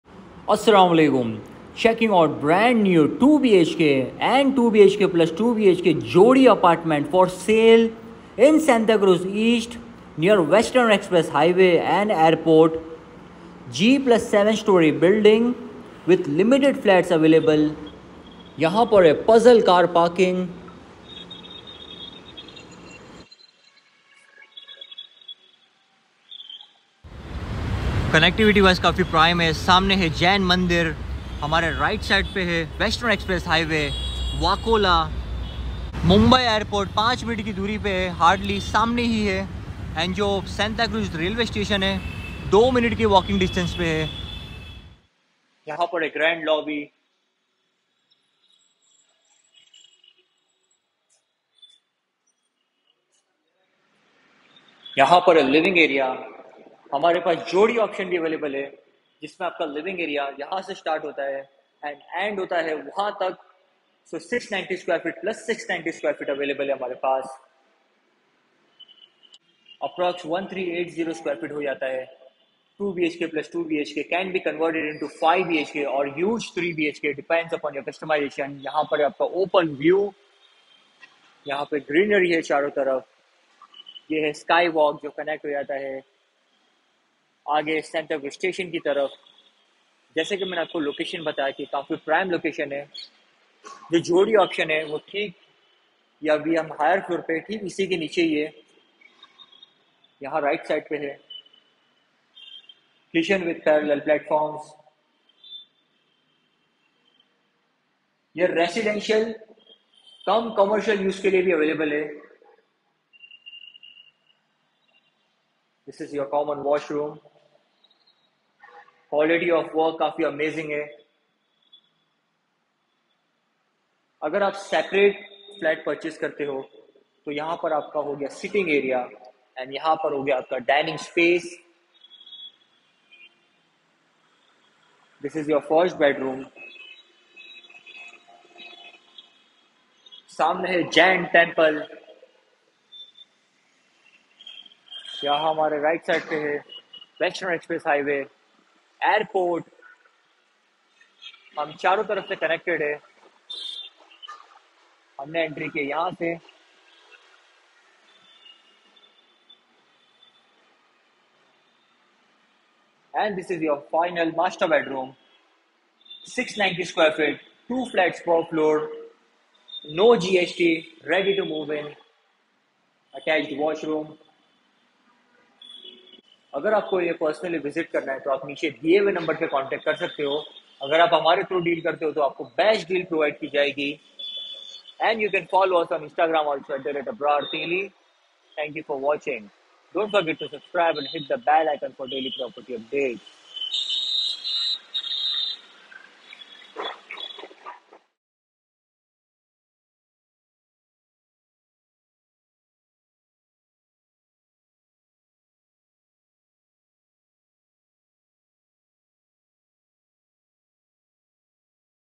Brand New 2bhk for sale 2.35crores near Airport Santacruz East
Brand new 2Bhk for sale in Santacruz East

Jodi options available.
2,75crores All inclusive nego

Good Connectivity

Airport is 5/7mins away
Western Express Highway is 30 seconds away
Railway station is 2mins away

Call for inspection:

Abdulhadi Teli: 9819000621
Raees Realty